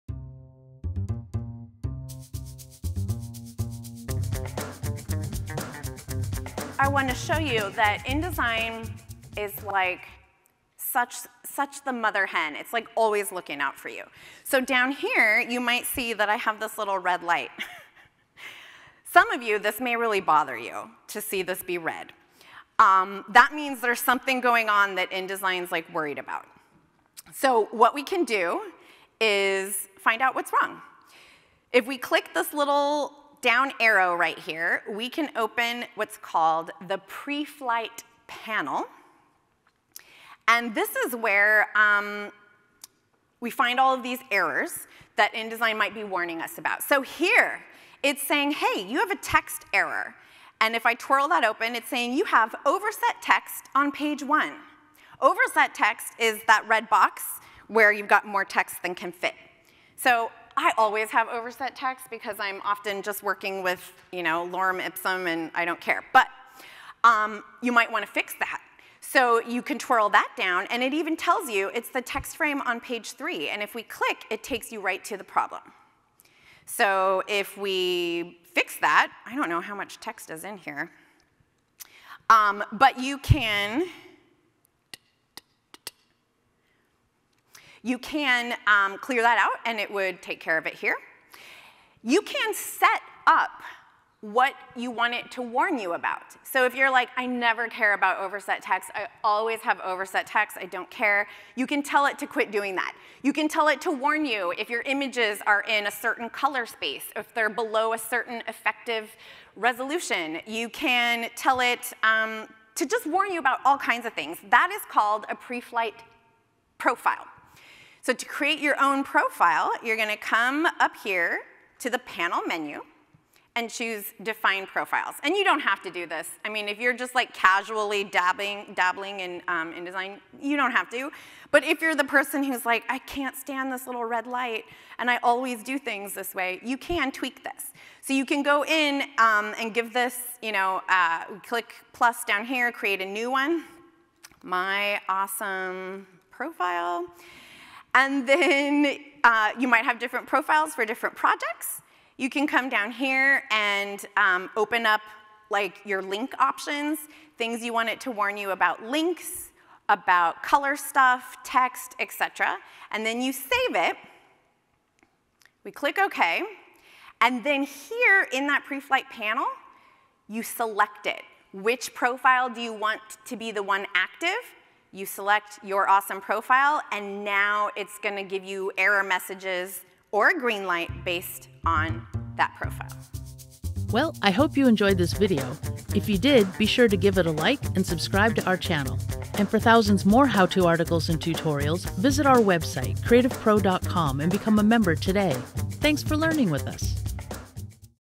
InDesign: How to Create Preflight Profiles (Video Tutorial)
In this how-to video, Khara Plicinic shows how to make sure your InDesign file fits within a set of parameters for what’s allowed. By setting up preflight profiles, you can, for instance, make sure that your file only allows a specific color mode, isn’t missing fonts, doesn’t have overset text, or doesn’t violate any other rules you put into place.